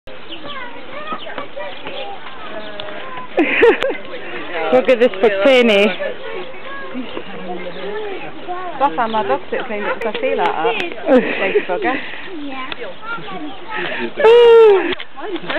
Meerkats
Meerkats in a zoo in Tunisia watching us watching them